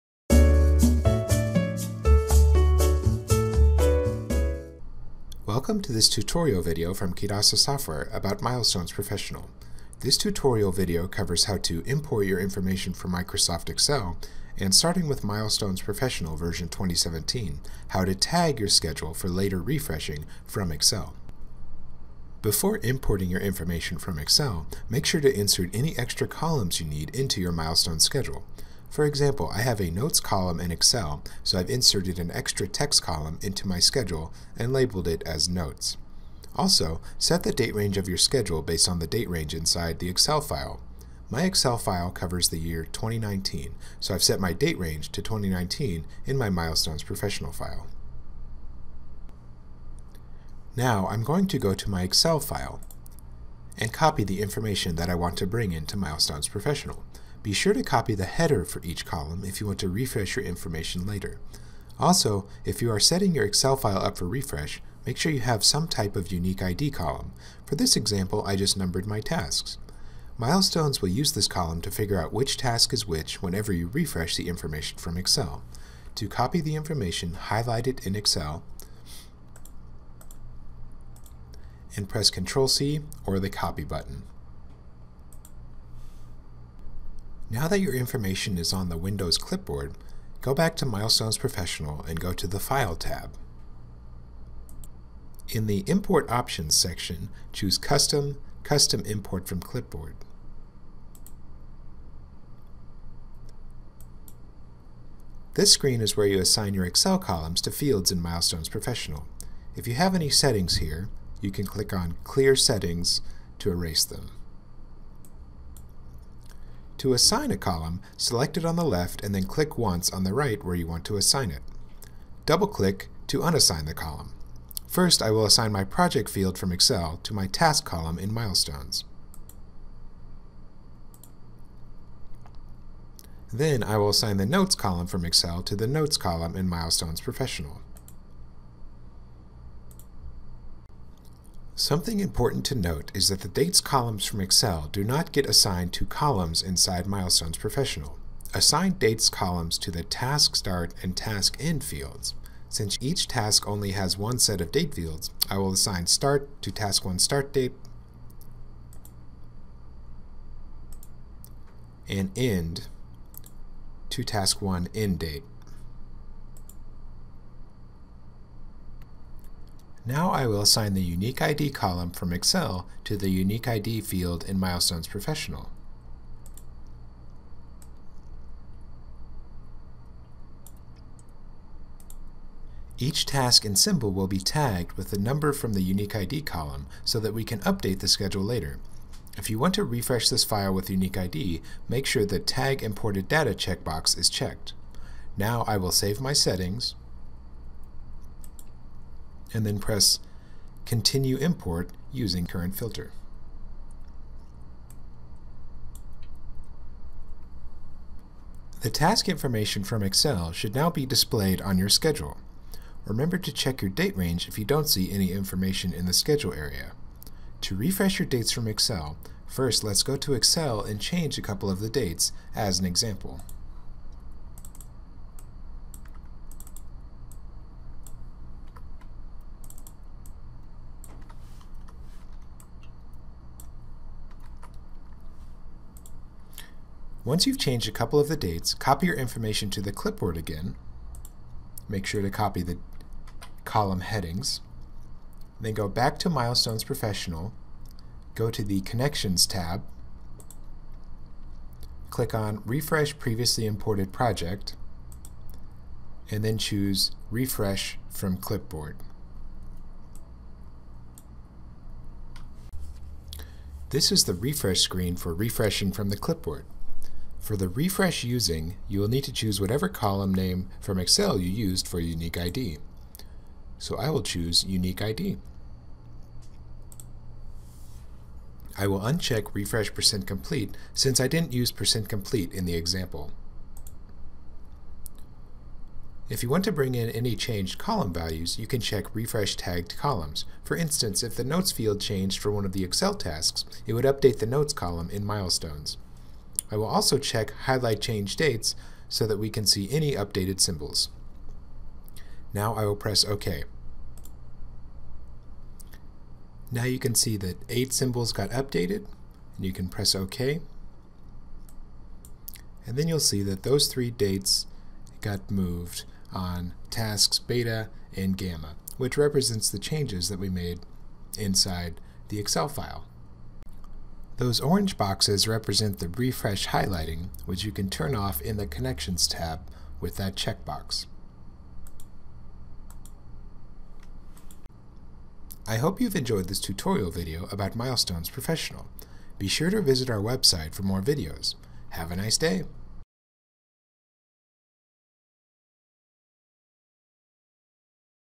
Milestones Professional Tutorial - Excel Import + Refresh
Learn how to import your information from Microsoft Excel into Milestones Professional. You can also link your file to the Excel file starting with Milestones Professional 2017